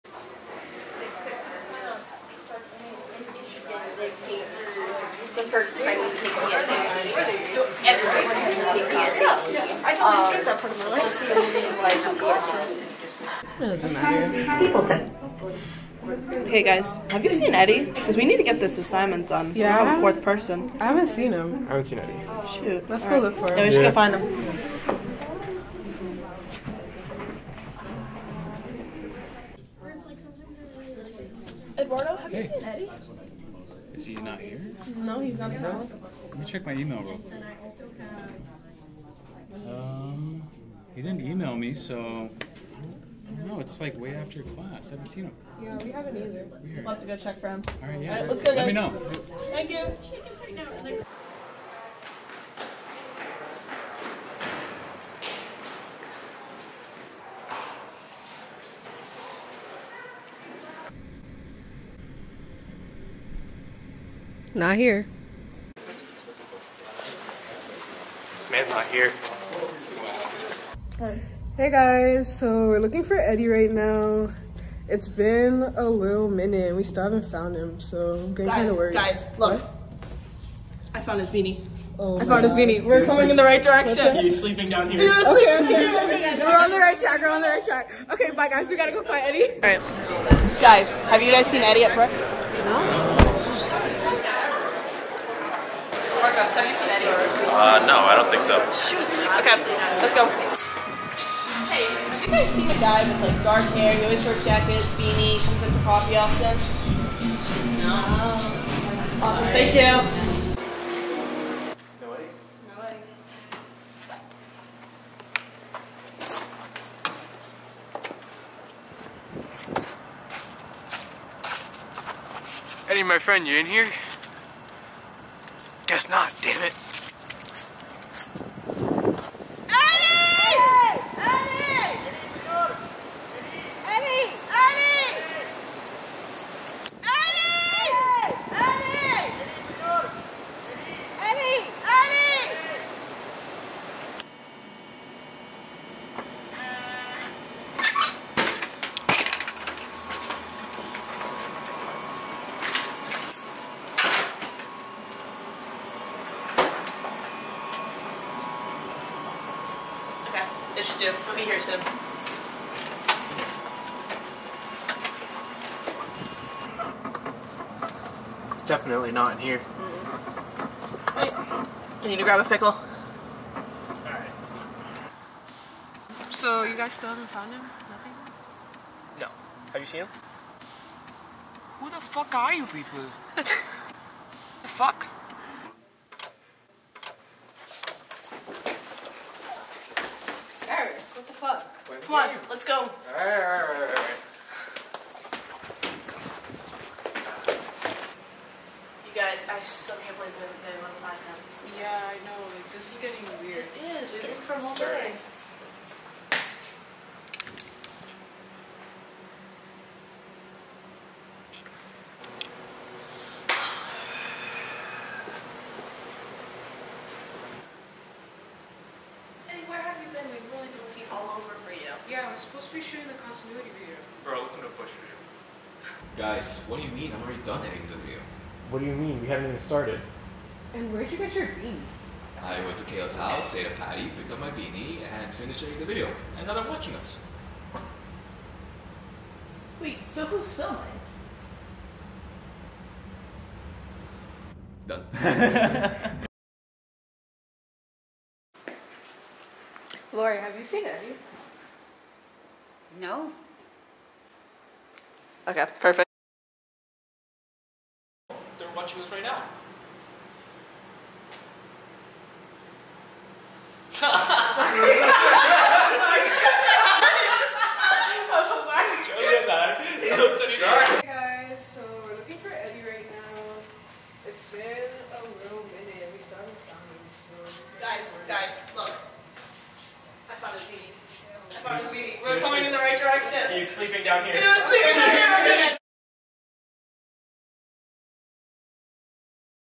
Finding Eddie
Student Film, RIT CAD 2023

By: Arik Motiani, Charli Rodriguez, Eduardo Garcia, Kayla Nugent

For Continuity assignment in 4D Design

Description:
When Arik, Charli, and Kayla are all partnered up for the Continuity assignment, the group quickly realizes their 4th member, Eddie is missing. They run all throughout campus on a hunt for Eddie looking for places he could be at.